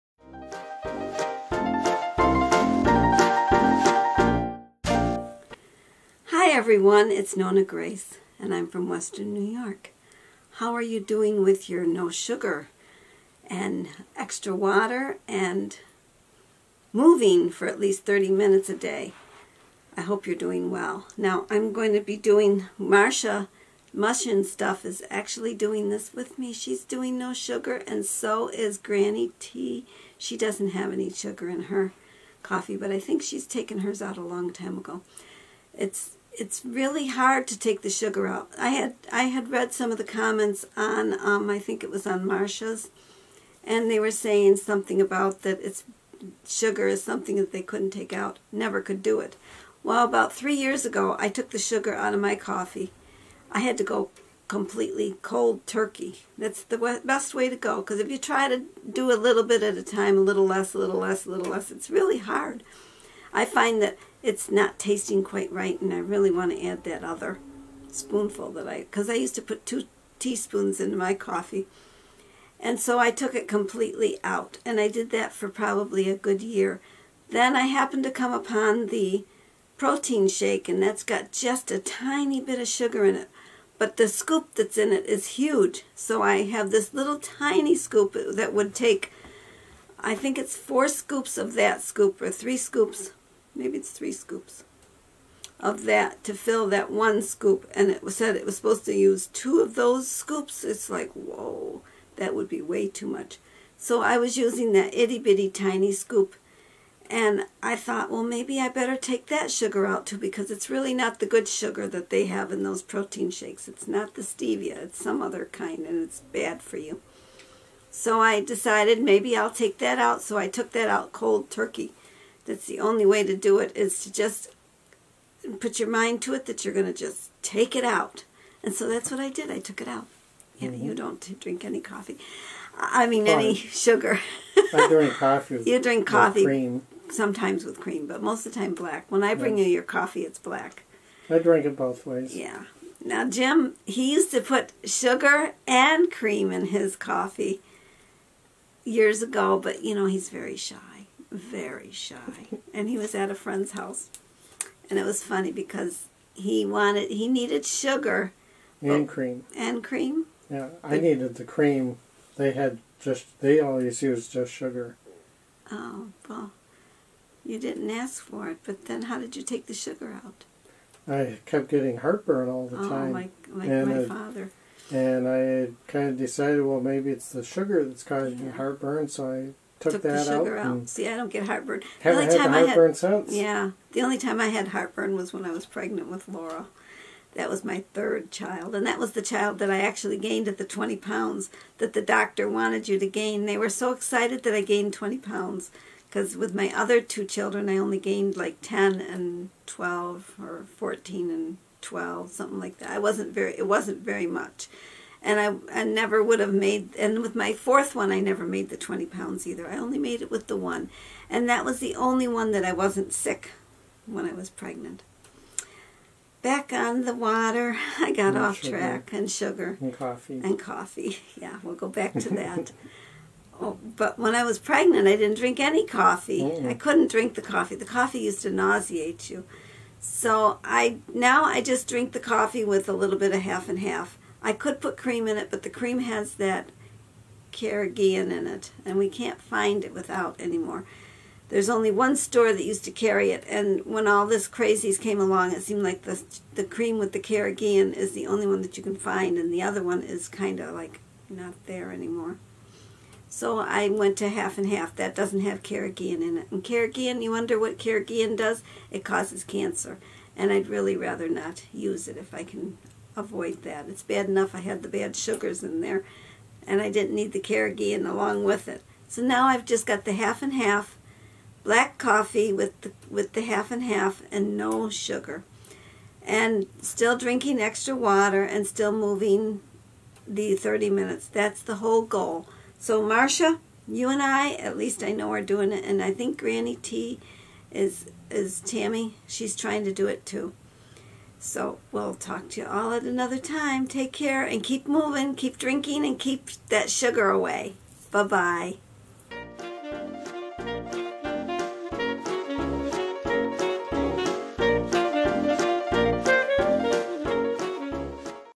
Time To Cut The Sugar
Time to cut the sugar, Marsha and I and Granny T are cutting the sugar in our  coffee. How about you time to cut the sugar out of your coffee.  Drink more water and more more at least 30 minutes
Marsha from: Marsha's Mush & Stuff and Granny T. from: .Life with Granny T